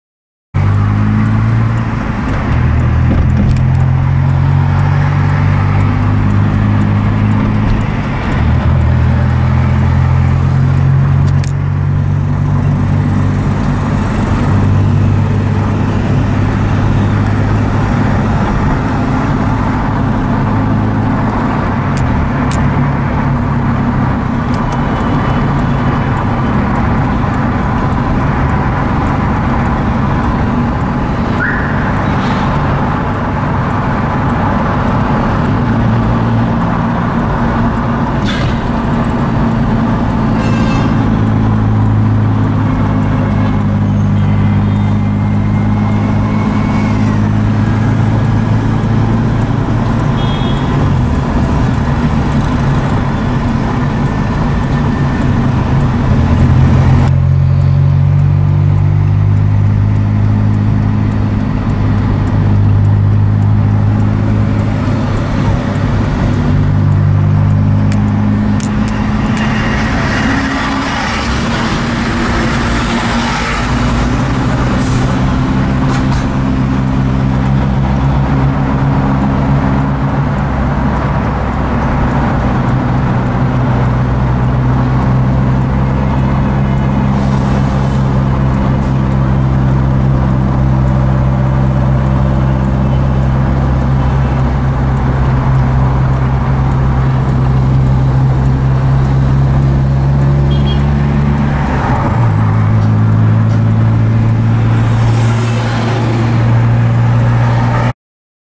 Pasando por el túnel del Paso a Desnivel de la Av San Juan en horas de la noche , al norte y al sur
Ahora  recorremos la avenida De los Héroes   pasando por el paso a  desnivel de la Av. San Juan, en el  distrito de San Juan de Miraflores en Lima -Peru, en horas de la noche . Jueves  22  de  abril del 2021.